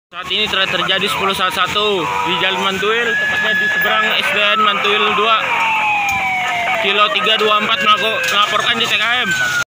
4 buah rumah kebakaran di📍Jl. Mantuil Permai Antasan Bondan, Kel. Mantuil, Banjarmasin
4 buah rumah kebakaran di📍Jl. Mantuil Permai Antasan Bondan, Kel. Mantuil, BJM Selatan. Rabu (19/05) sekitar jam 4 dinihari.